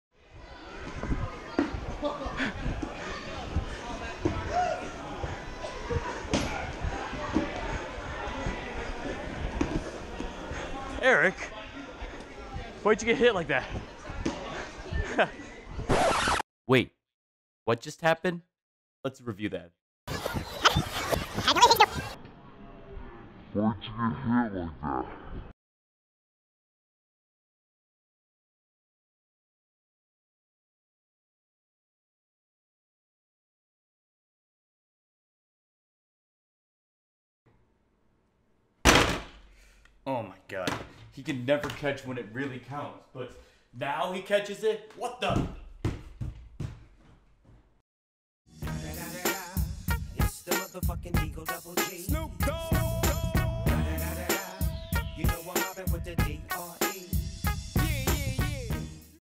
Eric makes the most casual one handed dodgeball catch ever!
Footage at the Sky Zone Fargo Trampoline Park.